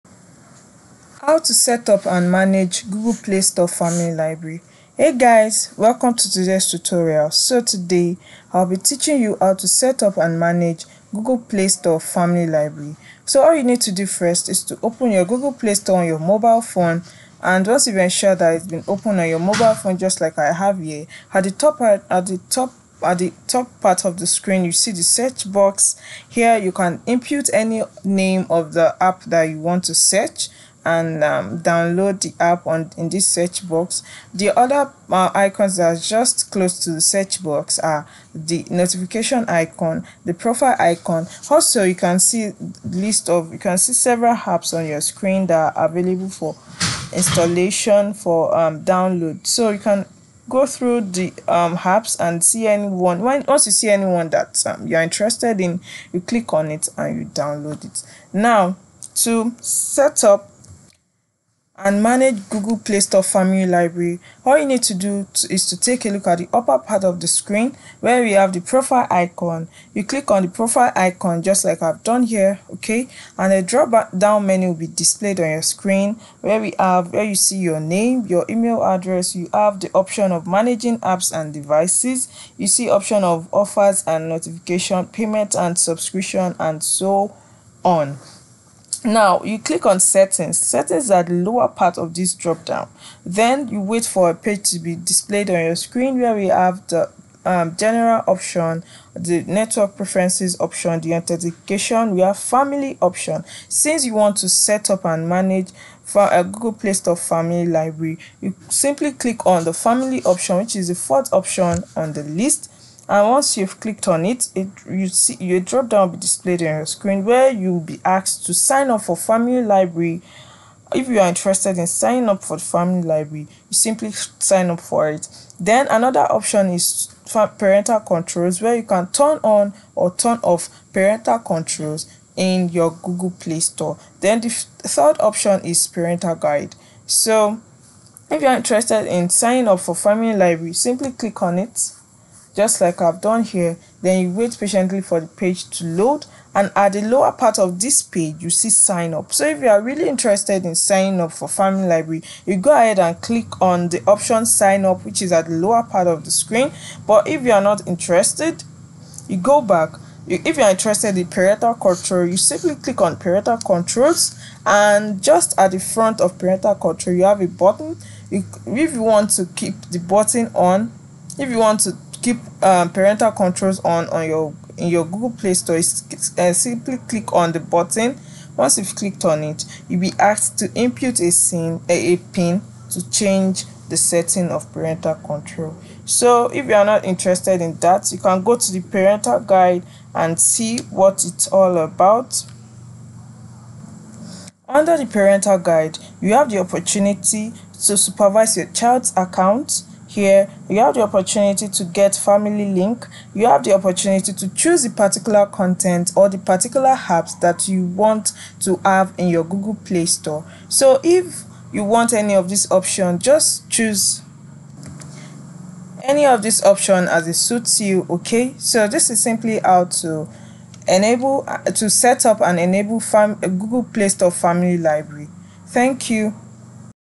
How to Set Up And Manage Google Playstore Family Libraby (Easiest Way)
Today we talk about How to Set Up And Manage Google Playstore Family Libraby, so stay until the end of the video to see the full explanation.